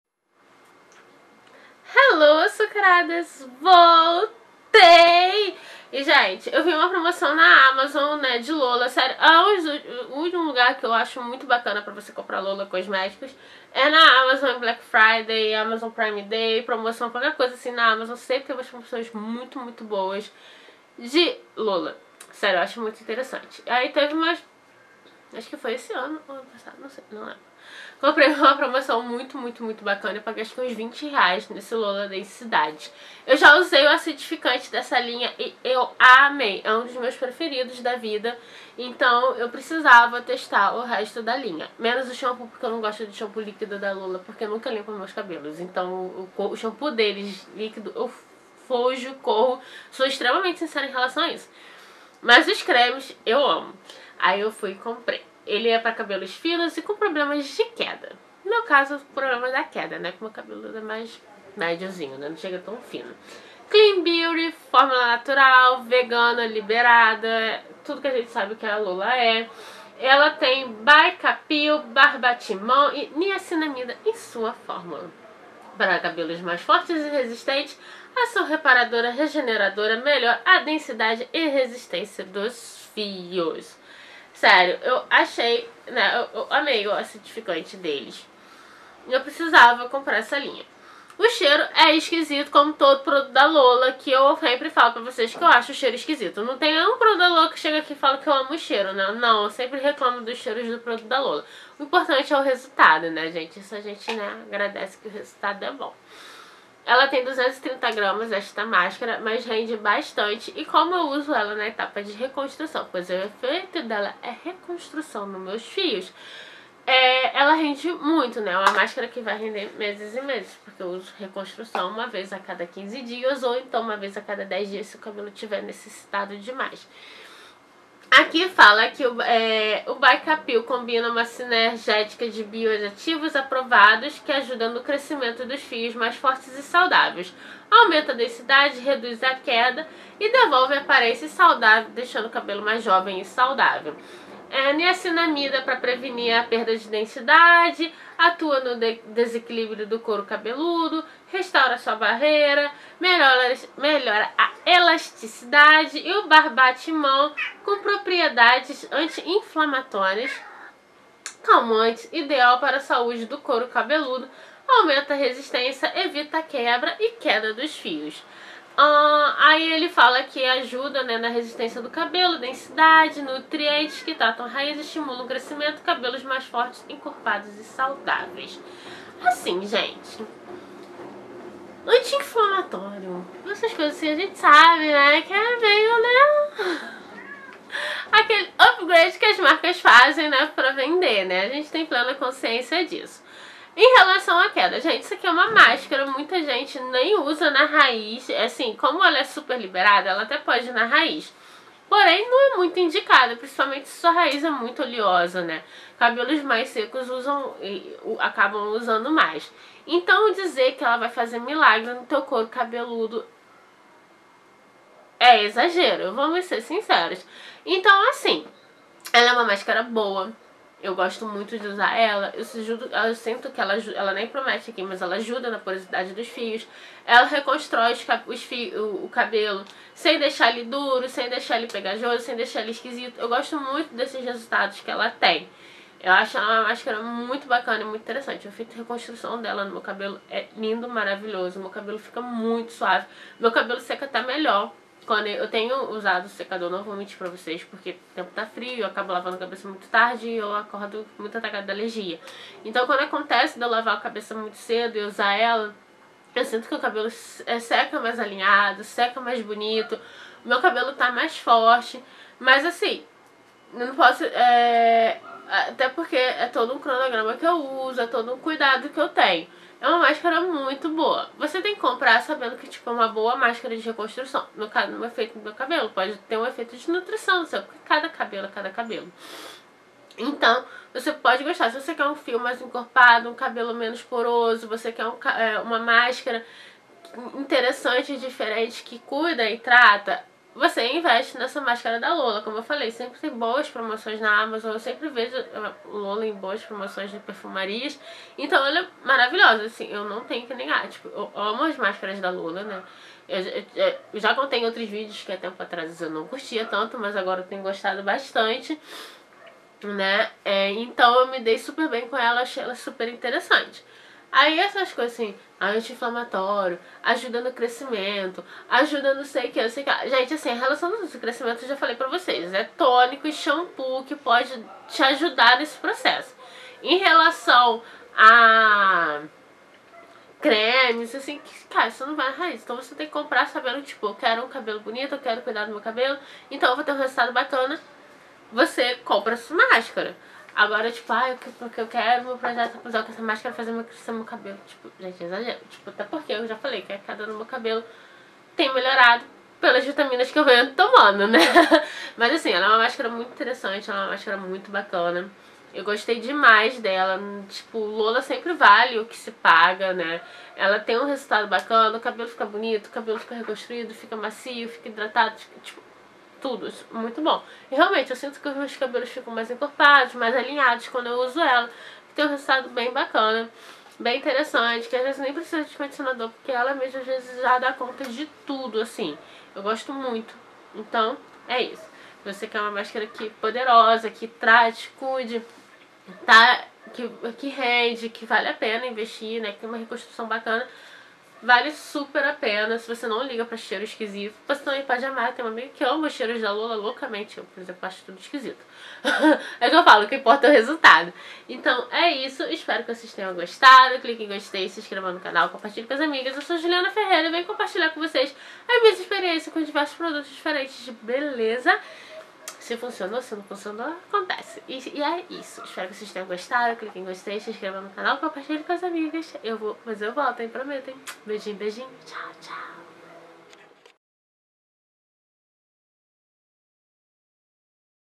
Máscara de Reconstrução Lola Cosmetics Densidade!
Hello Açucaradas,

E hoje trago minha experiência completa com a Máscara de Reconstrução Lola Cosmetics Densidade, venha conhecer seus benefícios e como foi a minha experiência com ela.

Beijos Açucarados!